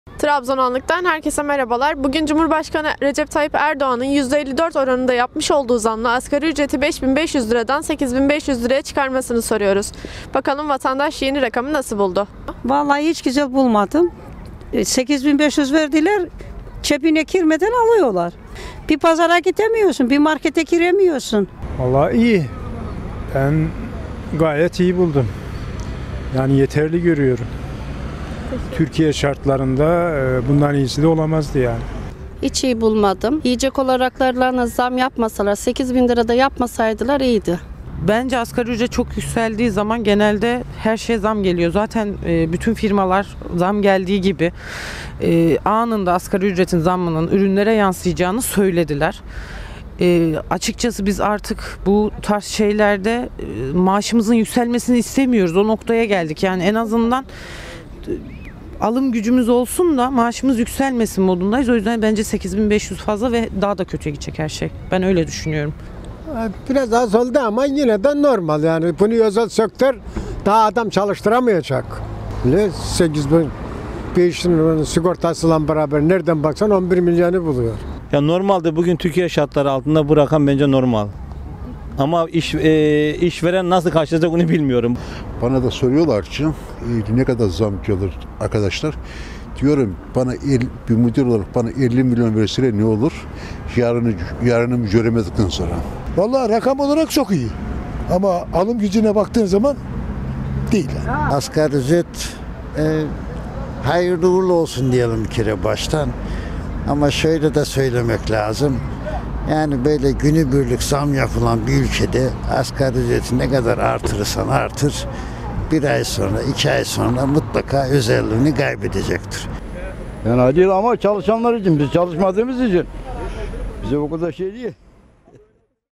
YENİ ASGARİ ÜCRET 8.500₺ HAKKINDA NE DÜŞÜNÜYORSUNUZ?
SOKAK RÖPORTAJ SERİMİZDE BUGÜN YENİ ASGARİ ÜCRET 8.500₺ HAKKINDA NE DÜŞÜNÜYORSUNUZ? SORDUK. Herkese İyi Seyirler.

☞ ASGARİ ÜCRET NE KADAR OLMALI?
☞ ATEİST BİRİYLE SEVGİLİ OLUR MUSUNUZ?
☞ KADIN OLMAK MI DAHA KOLAY ERKEK OLMAK MI?
☞ TRABZON'U 3 KELİME İLE ANLATIR MISIN?